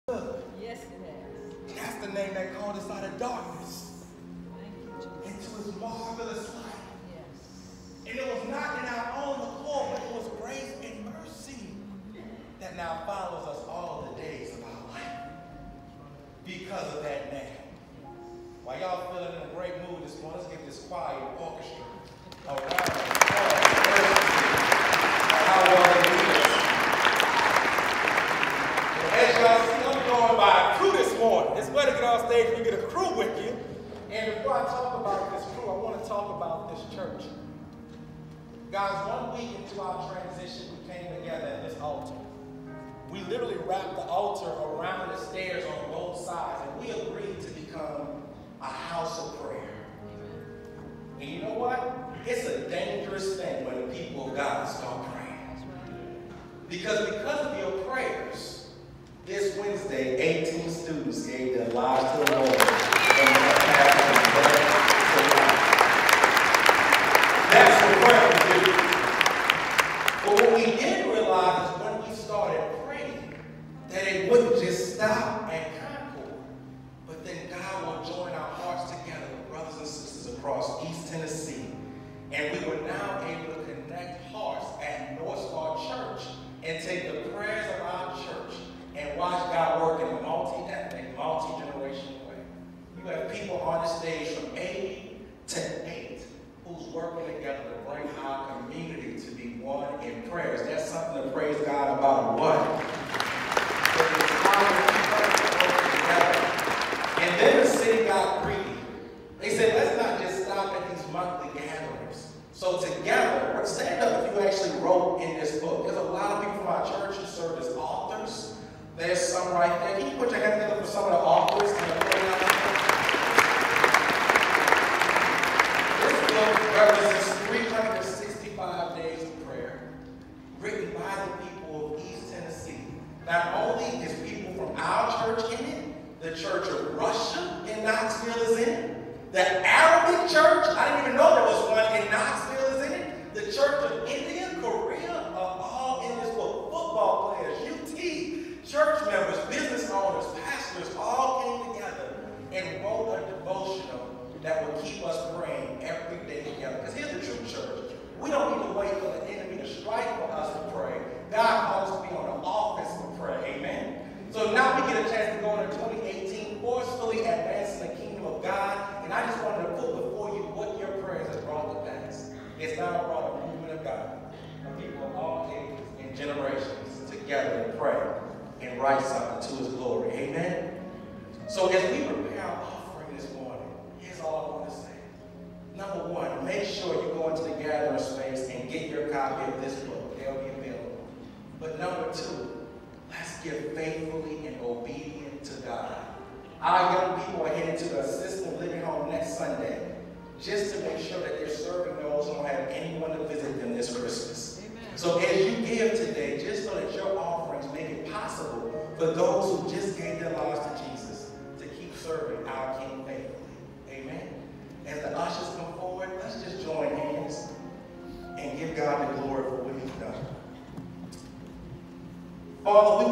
Pray Knox 2018
Andrew Osakue at First Baptist Concord in Farragut, TN explains the Pray Knox book for 2018.